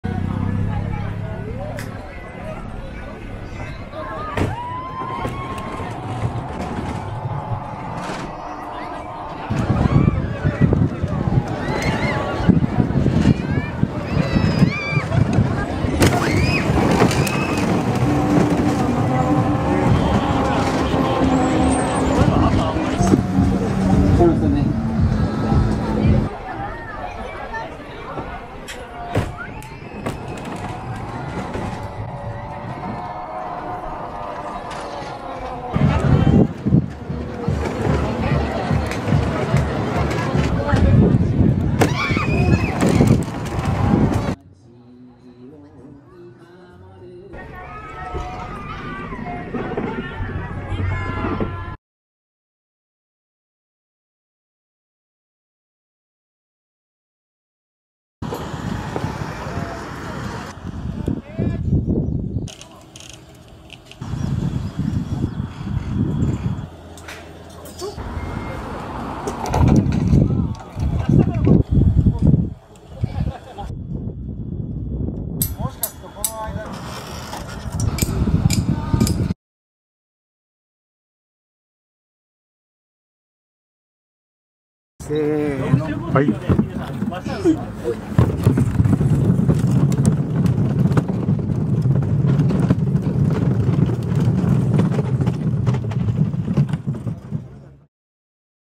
Bamboo coaster makes its lone annual appearance in Chiba Prefecture - The Japan News
For one day every year, the excited cries of riders on a handmade “roller coaster” sound out in a Chiba Prefecture town as airplanes from nearby Narita Airport fly in the distant sky. This year, the ride in the town of Tako made its appearance on June 9.